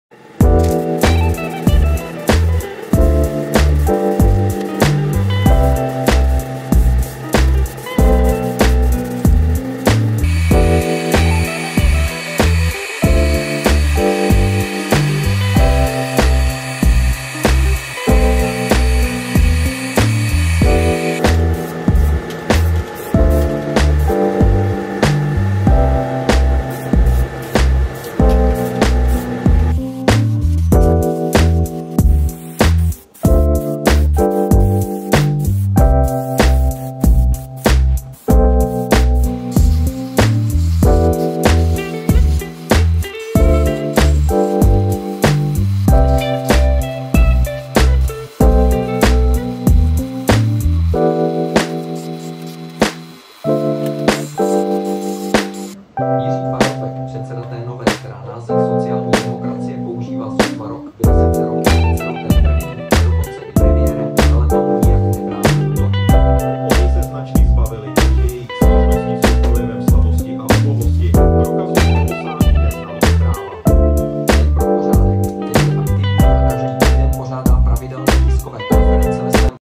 3d printed Carnage livesize bust part 4
I finally got around to painting and I already looked through and got the food colors and it's starting to look the way it should. Model by @Wekster .  Printed on @prusa3d Prusa XL from Prusament PLA.